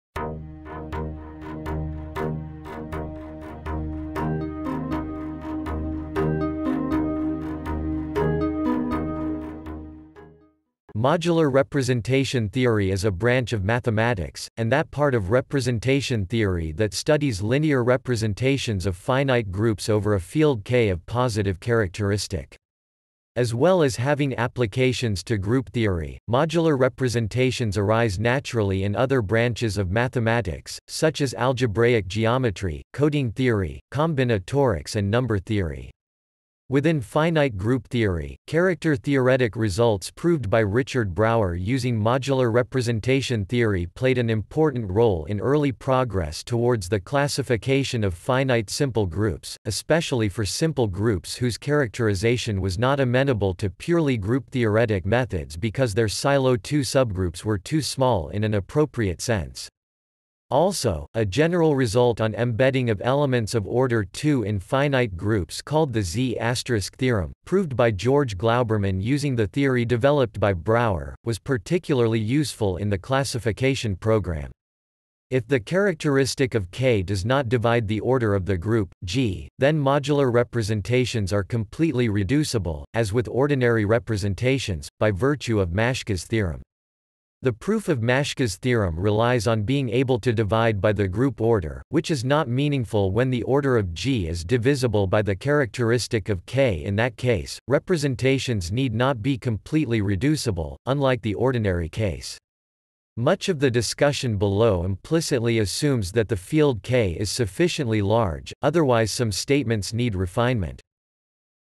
What is Modular representation theory?, Explain Modular representation theory
~~~ Modular representation theory ~~~

Title: What is Modular representation theory?, Explain Modular representation theory

Description: Modular representation theory is a branch of mathematics, and that part of representation theory that studies linear representations of finite groups over a field K of positive characteristic. As well as having applications to group theory, modular representations arise naturally in other branches of mathematics, such as algebraic geometry, coding theory, combinatorics and number theory. Within finite group theory, character-theoretic results proved by Richard Brauer using modular representation theory played an important role in early progress towards the classification of finite simple groups, especially for simple groups whose characterization was not amenable to purely group-theoretic methods because their Sylow 2 subgroups were too small in an appropriate sense. Also, a general result on embedding of elements of order 2 in finite groups called the Z* theorem, proved by George Glauberman using the theory developed by Brauer, was particularly useful in the classification program. If the characteristic of K does not divide the order of the group, G, then modular representations are completely reducible, as with ordinary  representations, by virtue of Maschke's theorem. The proof of Maschke's theorem relies on being able to divide by the group order, which is not meaningful when the order of G is divisible by the characteristic of K. In that case, representations need not be completely reducible, unlike the ordinary case. Much of the discussion below implicitly assumes that the field K is sufficiently large , otherwise some statements need refinement.